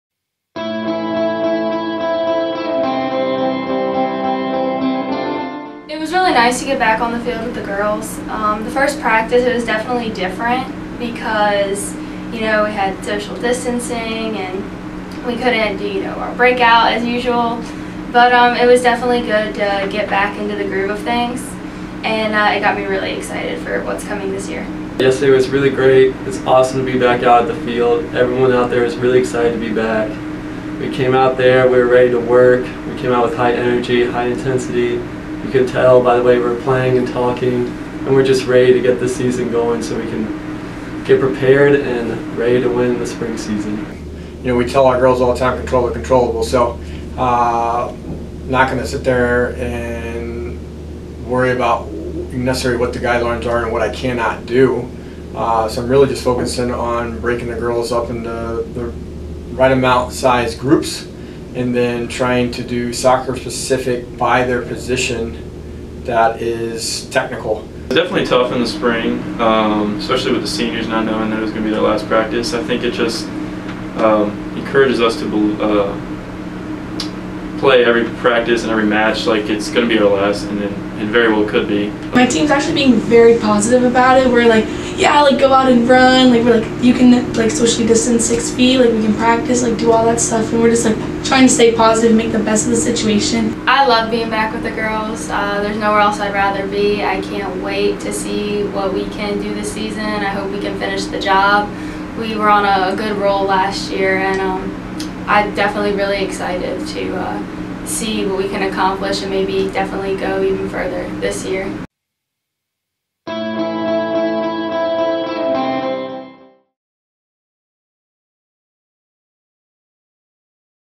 Eagles Rise: Week Seven
Bring in the coaches! Week seven marks the official start of practices for the Eagles.